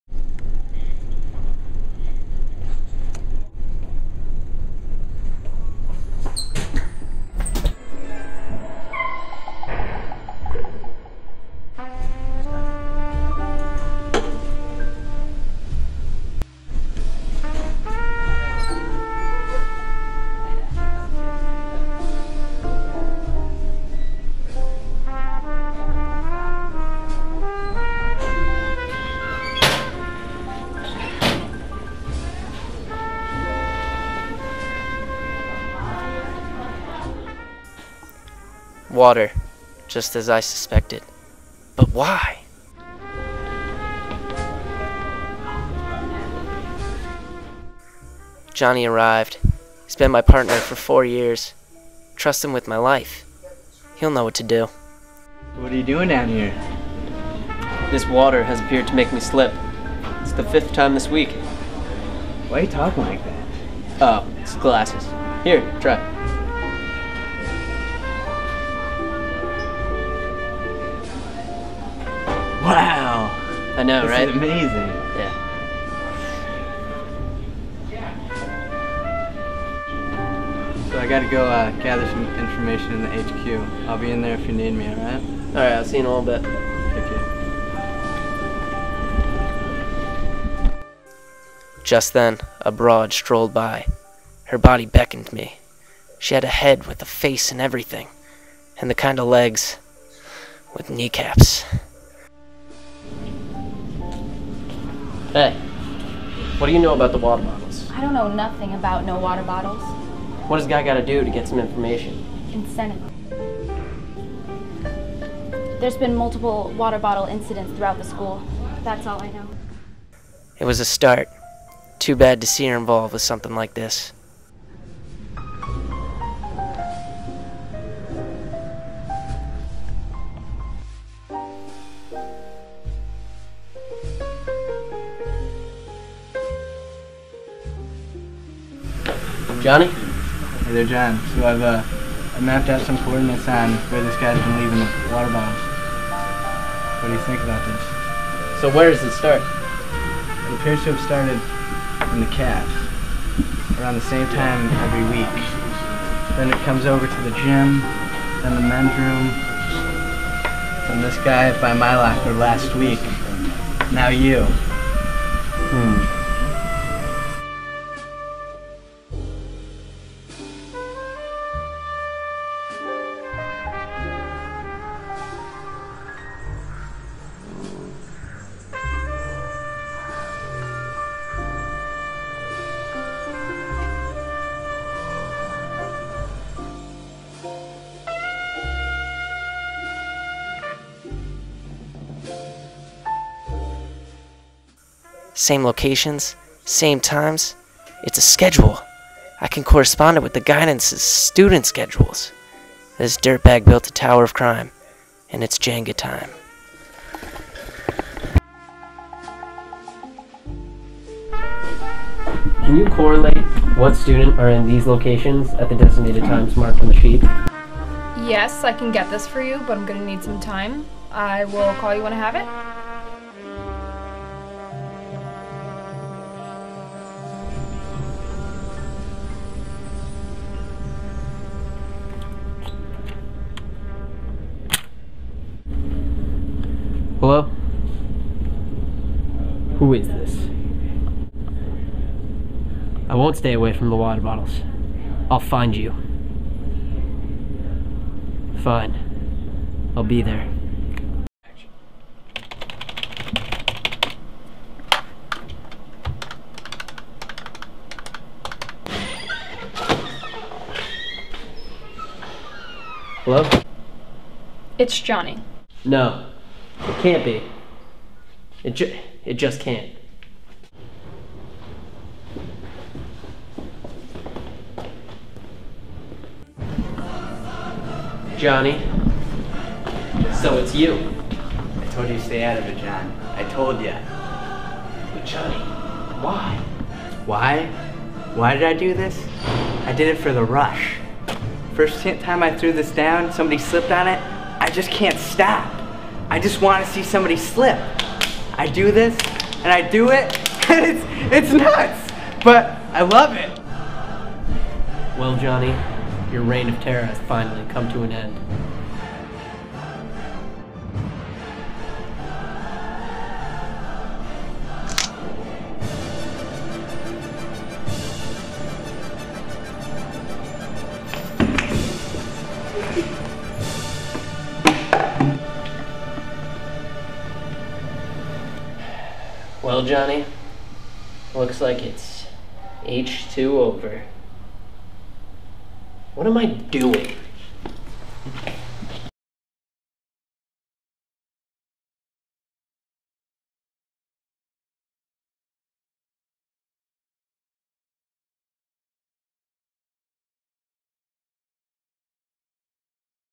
Four Bestfriend Final Film **Film Noir**
Detective John Bennett is on the case when the water bottle terrorist strikes close to home. Nothing can stop him from piecing together this crime. Along with his trustworthy partner, and the information of a temptress he will try to find out what is going on.

Special Thanks to:
Monica Felix
Mrs. B
and  of course the great
Mr. E